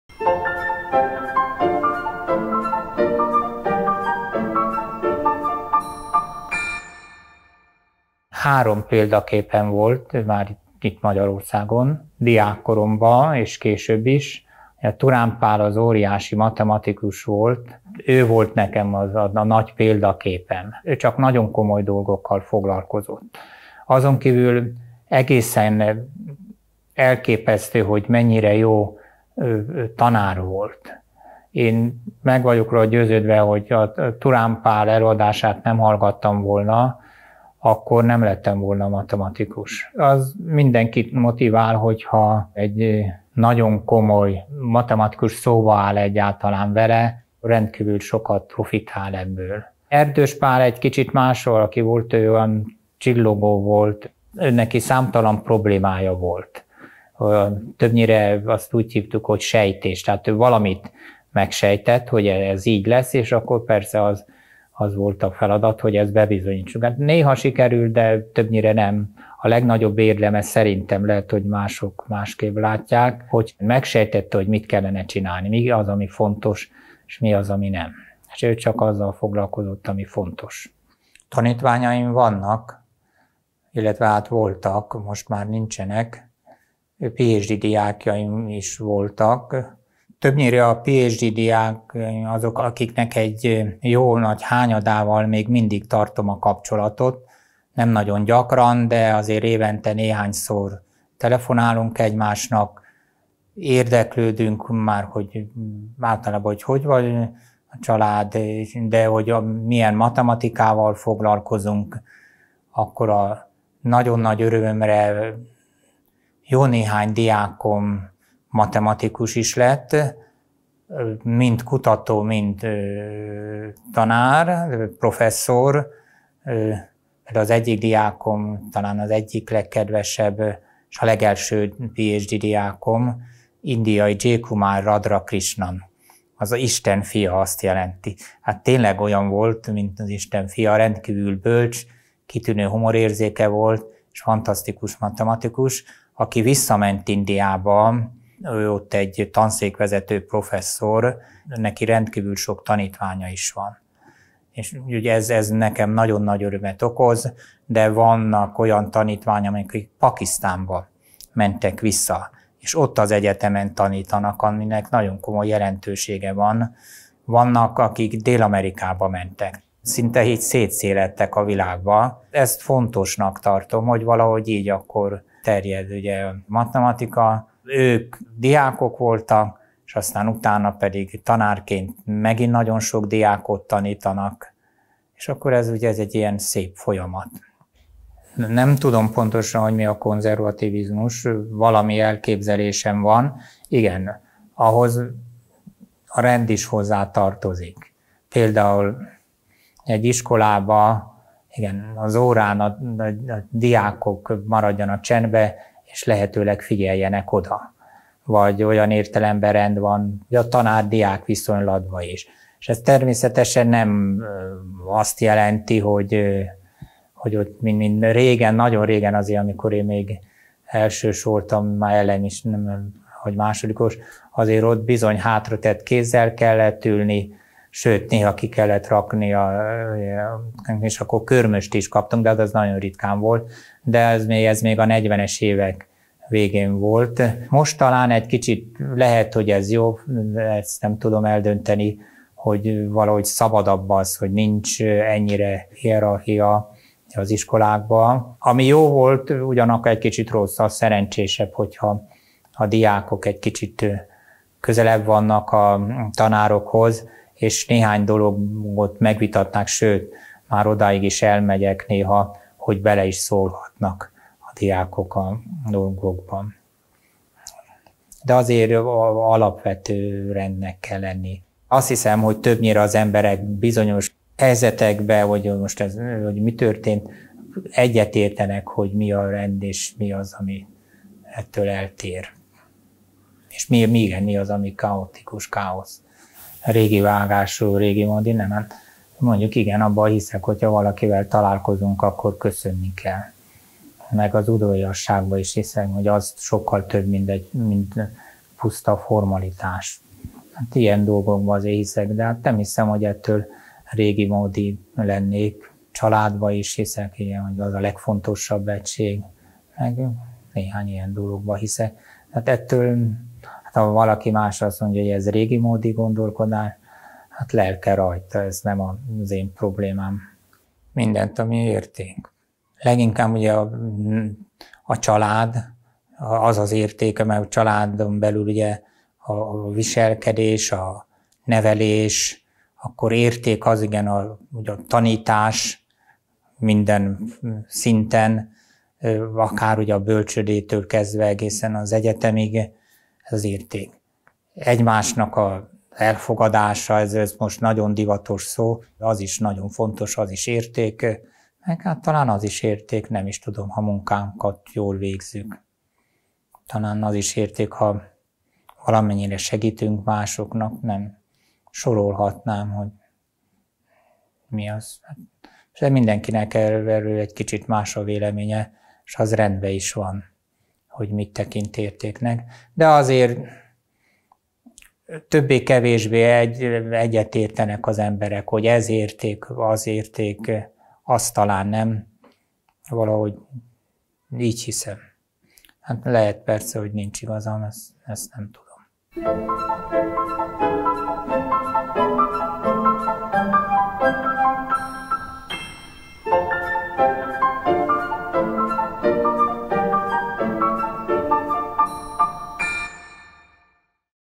Tükör-Képek / Szemerédi Endre
SZEMERÉDI ENDRE
Abel-díjas matematikus

„Nem tudom pontosan, mi a konzervativizmus, de a rend biztos hozzátartozik. Például egy iskolában a diákok lehetőleg maradjanak csendben és figyeljenek oda. Ez persze nem vasszigort jelent, de kölcsönös tiszteletet feltétlenül. Különben azt hiszem, az emberek nagyjából egyetértenek abban, mit tekintünk rendnek, és mi az, ami ettől eltér: a káosz.”

„1940-ben születtem Budapesten. Az Eötvös Lóránd Tudományegyetem matematikus szakán végeztem, az érdeklődésem különösen a diszkrét matematika, a kombinatorika és a gráfelmélet területére terelt. A matematika tudományában mesteremnek tartom Turán Pált, Erdős Pált és Hajnal Andrást. 1990-ben kaptam egyetemi tanári megbízást az egyesült államokbeli Rutgers Egyetem számítógép-tudományi tanszékén, közben párhuzamosan 1965-től a Magyar Tudományos Akadémia Rényi Alfréd Matematikai Kutató Intézetének kutatója voltam. Abban, hogy 2012-ben elnyertem az Abel-díjat, sokat köszönhetek a feleségemnek, Kepes Annának, aki türelmével és szarkasztikus humorával számtalan lelkileg nehéz pillanatban segített át.”